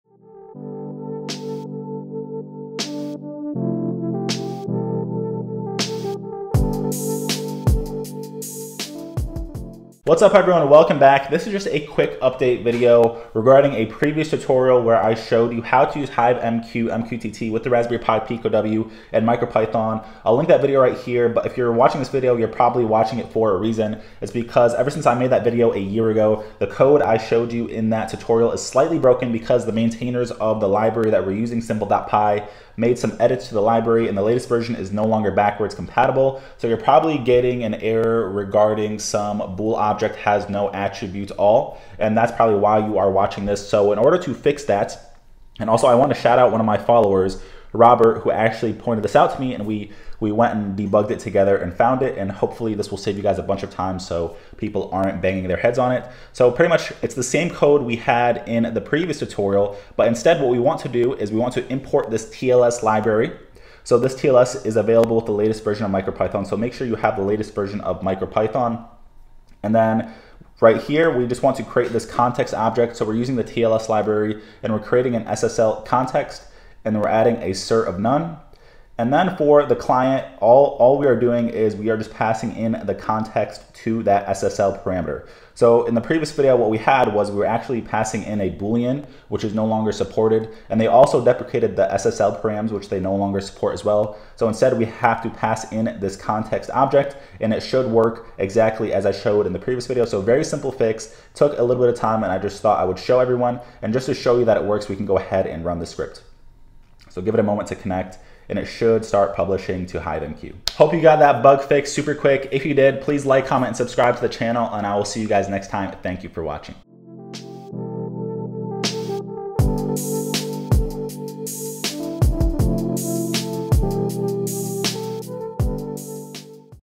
Connecting Raspberry Pi Pico W's with MQTT: Building a Collaborative IoT System (2024 Update)
Quick fix on a previous video where I showed you how to use HiveMQ with the Pico W in MicroPython. There has been a library update and I thought I would show you the bug fix.